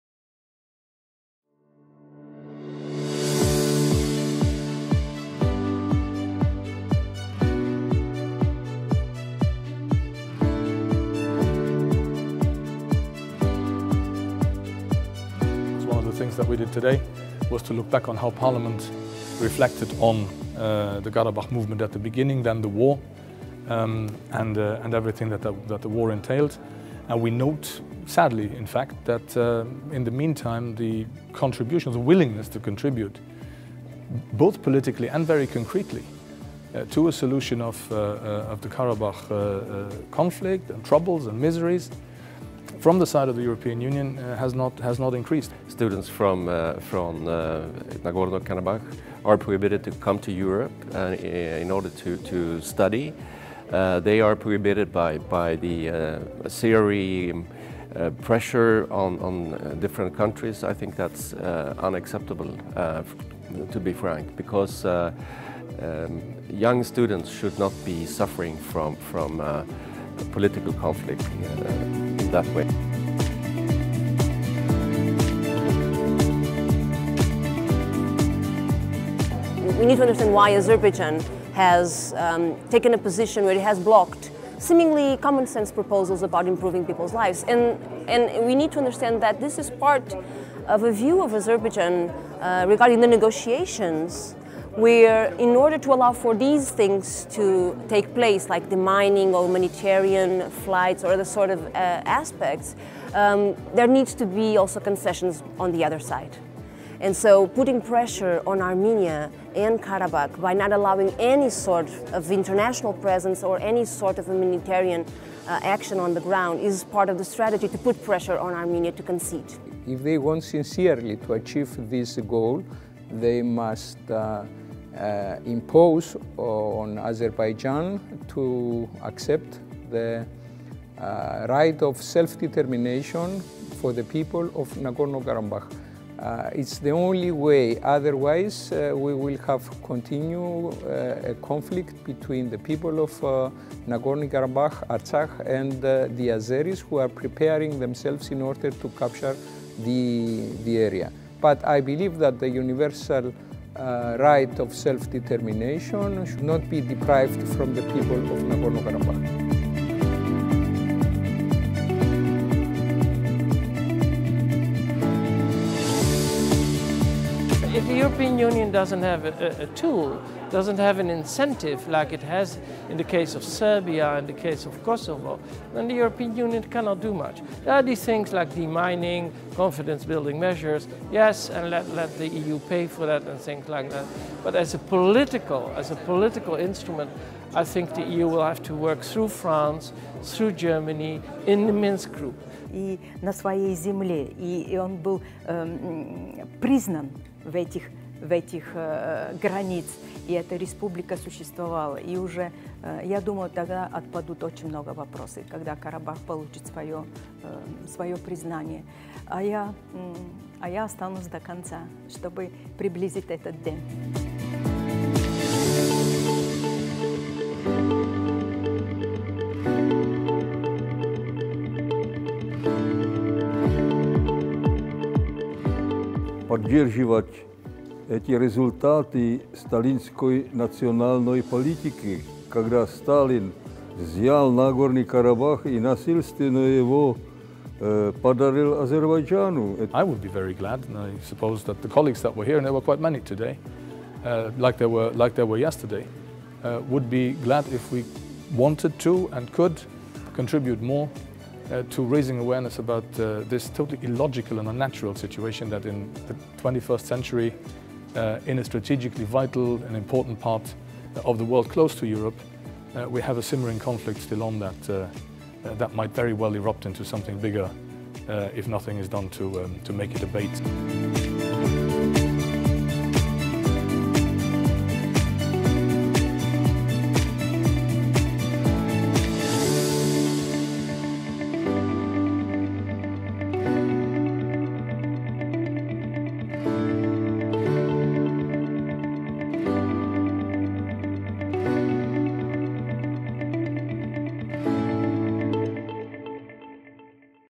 Nagorno-Karabakh & the EU: 1988/2018
A short video summary of the conference that AGBU Europe & European Friends of Armenia, EuFoA organized in the European Parliament on the occasion of the 30th anniversary of the Karabakh Movement.